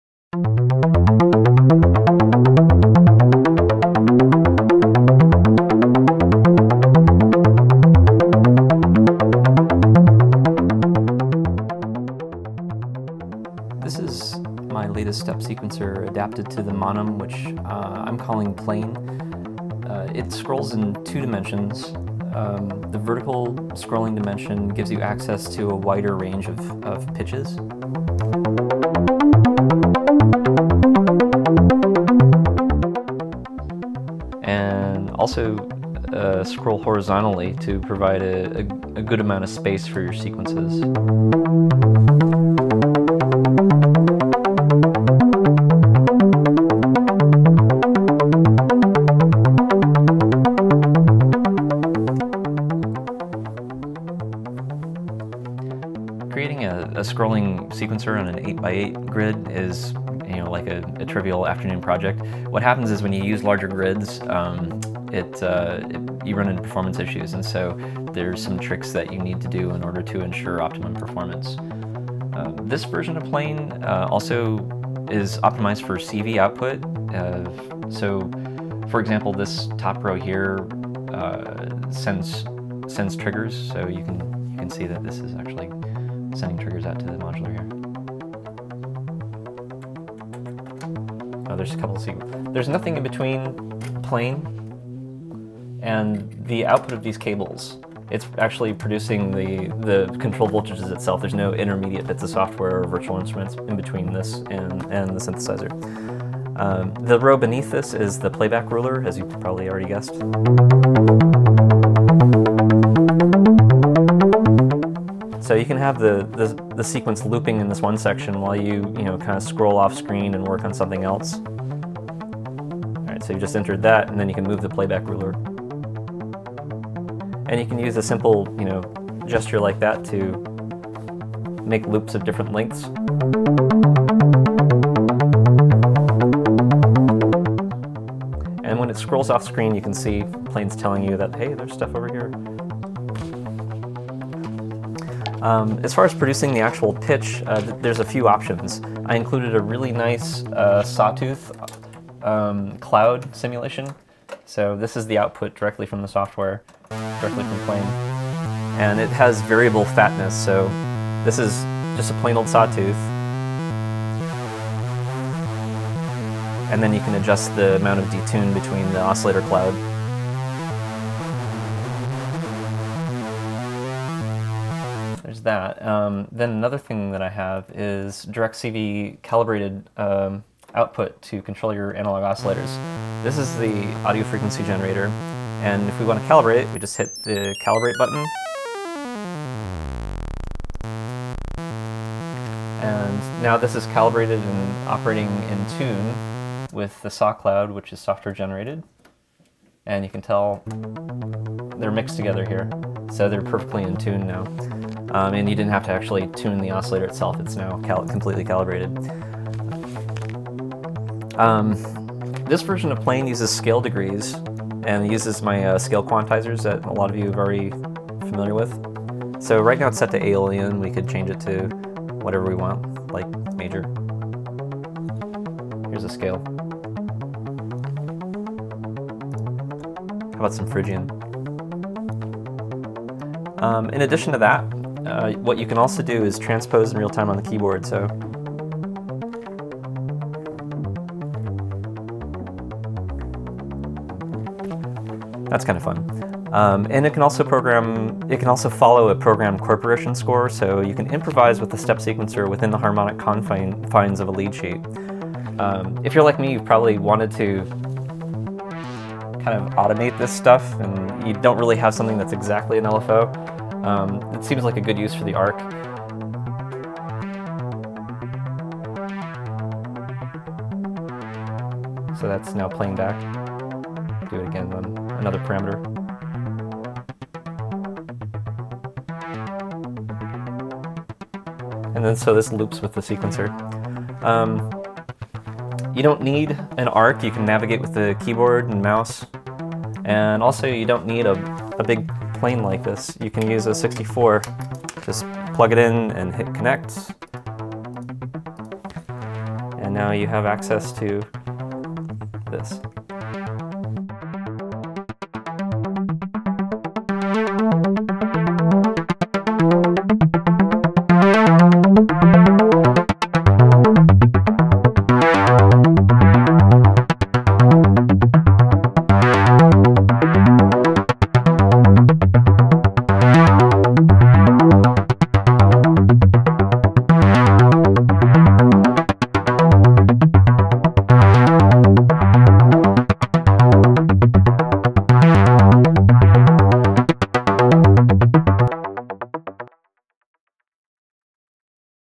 plane
hardware: monome 512, monome 64, monome arc4
software: plane

This video is the version with the voice over and saw cloud / osc calibration routine in the middle restored.

I've always loved step sequencers and I see the monome as an opportunity to address some of the grey area between the one knob per function analog step sequencer and step sequencers with memory. The idea is to increase the available note range without sacrificing precision and increase the available sequence length range, without sacrificing direct manipulation and feedback. So, when the arc came around it seemed like a useful navigational tool to manipulate a large plane of data.

I've been referring to plane as a platform because there are a number of variations I want to implement using the underlying development. This version is optimized to serve as a control voltage source. As such, it produces a lot of outputs. The top row is step enable/disable which is typically used to fire off envelopes. Plane is generating control voltages directly. There are no intermediate bits of software or virtual instruments in-between plane and the end of the patch cords controlling the modular.

The row underneath it the playback loop ruler. Pressing anywhere in the ruler area moves the playback loop to that location. A chorded gesture changes the loop size. One section of the sequence can be edited while playback is occurring elsewhere.When the loop ruler goes off the visible edge of the grid, it lets you know in which direction the active stuff is happening.

Included is a very nice saw cloud simulation of multiple detuned oscillators with adjustable fatness which you can plug directly into your modular.

Also included is direct, accurate CV control of an oscillator via a closed-loop calibration procedure. Of course, MIDI output is also available.

This version of plane uses scale degrees and passes though my modal scale quantizer, so you can switch scales on the fly. You can also use a MIDI keyboard to transpose.

Or, it can also follow a programmed chord progression score, allowing you to improvise with a step sequencer within the harmonic framework of a lead sheet.

The bottom two encoders on the arc are serving as looping automated CV sources.

You don't have to use an arc with plane, you can navigate with the mouse or keyboard or powermate. Also, you don't need a 512 monome. All monome sizes are directly supported and can be hot swapped.